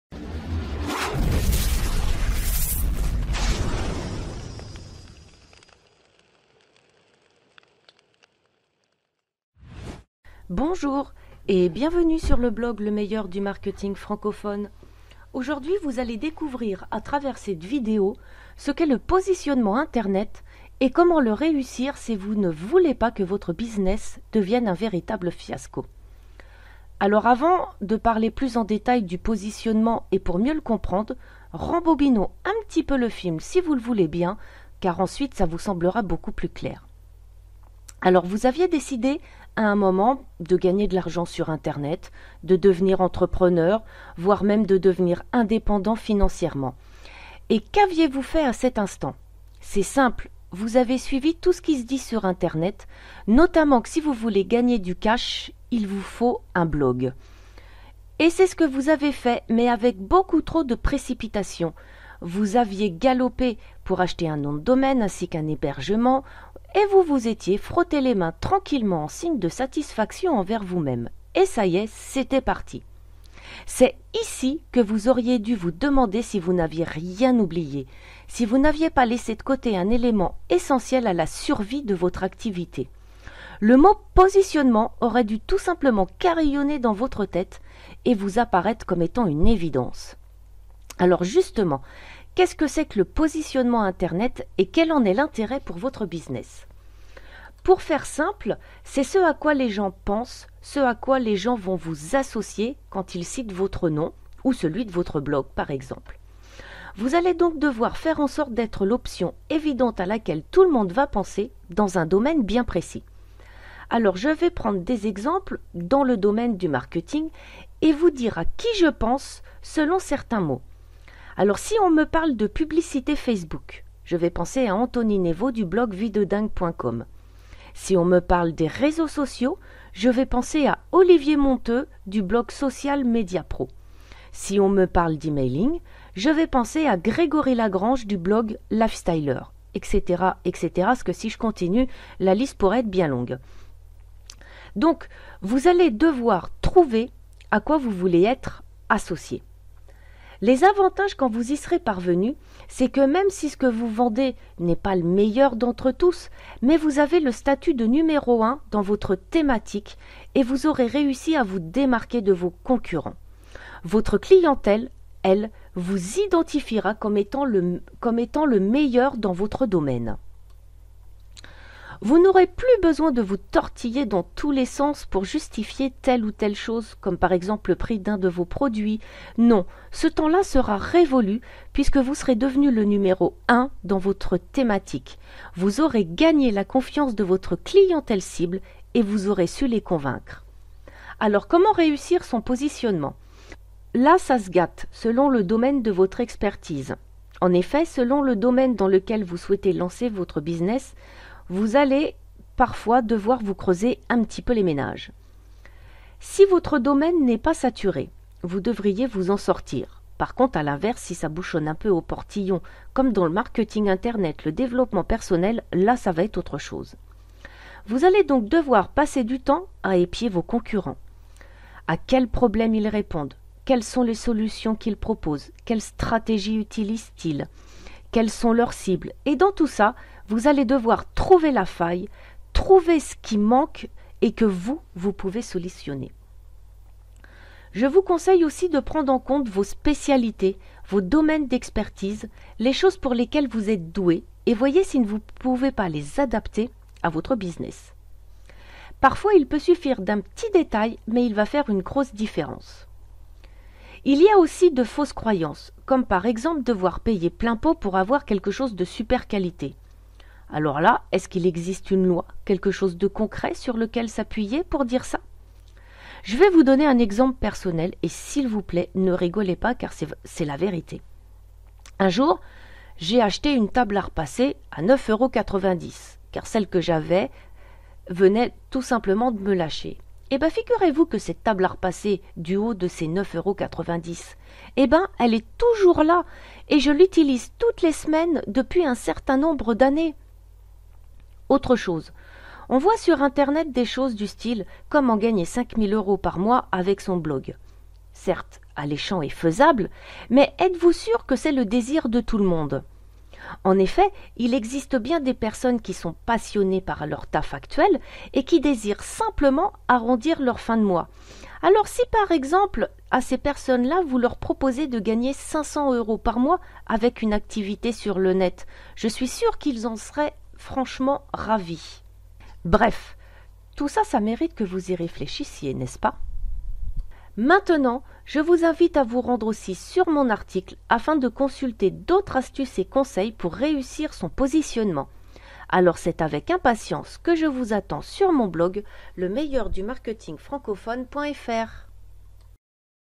Le positionnement : ça peut être rasoir mais ça peut aussi sauver votre business
Vous savez quel est le meilleur moyen pour échouer sur le web ? Vous savez pourquoi 99% des marketeurs jettent l’éponge ?

La réponse tient en un seul mot : positionnement

Ca peut paraître un peu chiant mais le positionnement stratégique est la base de votre business, si vous passez à côté de cette étape il y a de grande chance pour que vous fassiez partie des 99% des marketeurs qui se prennent une gamelle.

Vous devez absolument gardez à l’esprit que grâce au positionnement, tout le reste de votre business va couler comme de l’eau de roche : votre avatar client, la création de vos contenus, voir même la création de vos produits, en gros, il va vous permettre d’établir votre stratégie marketing et d’avoir un plan marketing en béton … vous pourriez même ne plus avoir à justifier vos tarifs.

Maintenant, je vous invite à en savoir plus et découvrez :
- qu’est-ce que le positionnement internet
- quel en est l’intérêt pour votre business
- comment réussir son positionnement
- quelles questions devez-vous vous poser par rapport à vos concurrents
- les fausses croyances qui peuvent vous permettre d’avoir un positionnement marketing en béton
- d’autres astuces et conseils du web pour parvenir à déterminer votre positionnement

Prenez en mains votre positionnement et devenez le numéro un de votre thématique.